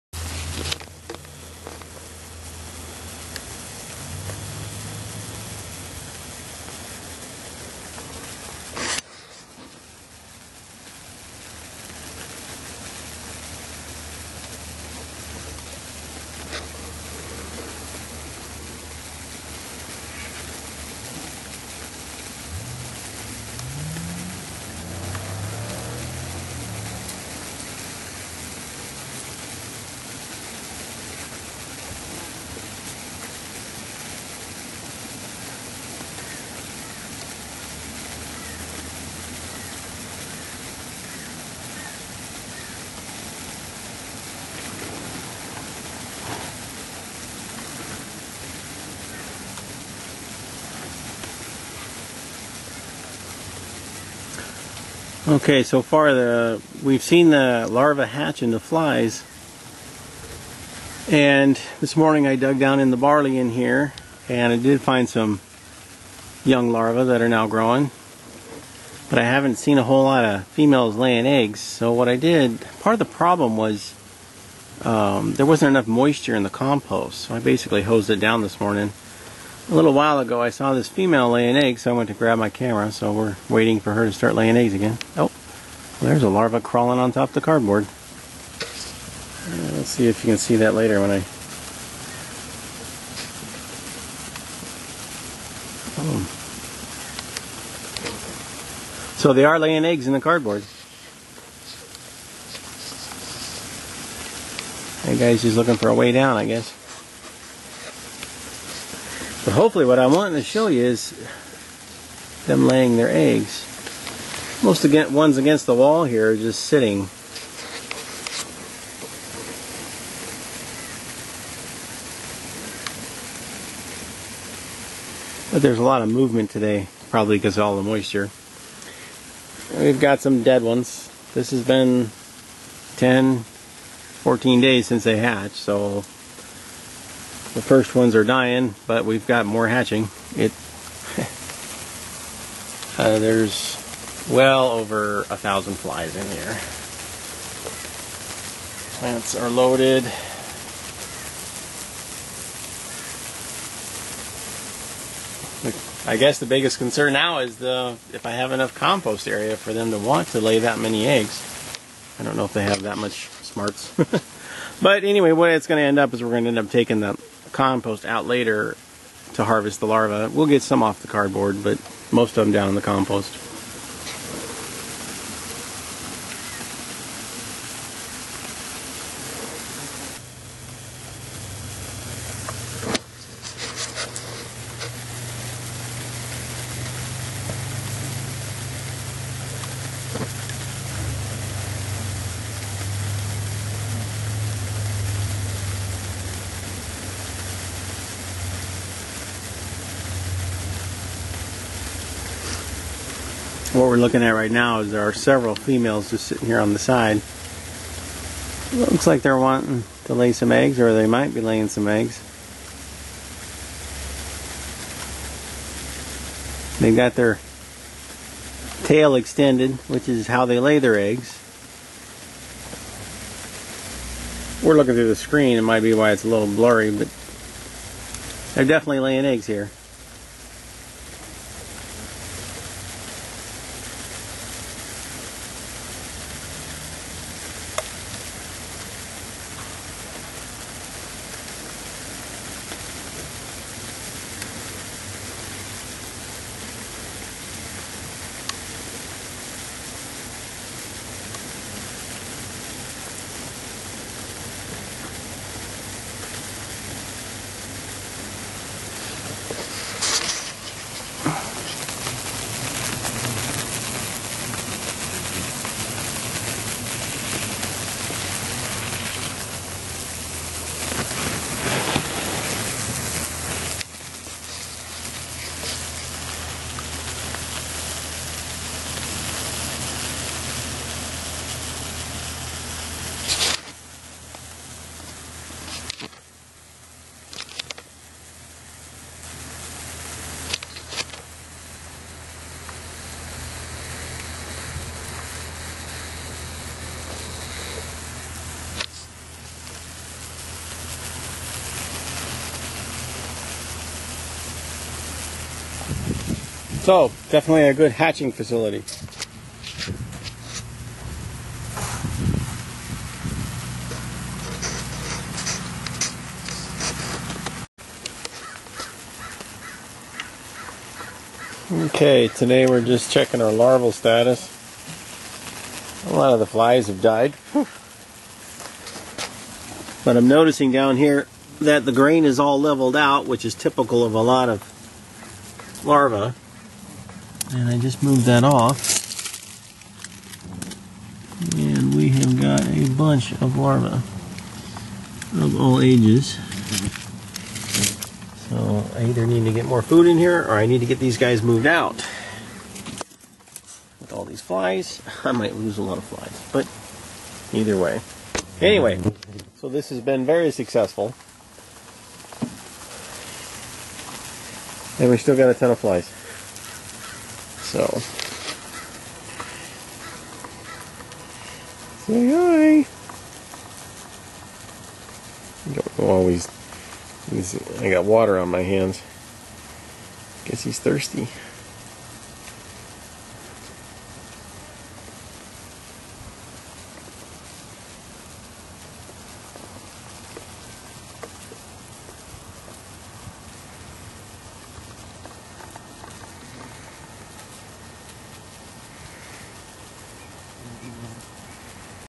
Black soldier flies laying eggs and a new mass hatch of larvae
When the conditions are right - Moisture, temperature, and food availability the flies will lay eggs and the hatch will be quick. You can order BSF Larvae on the website northwestredworms.com in the compost products section.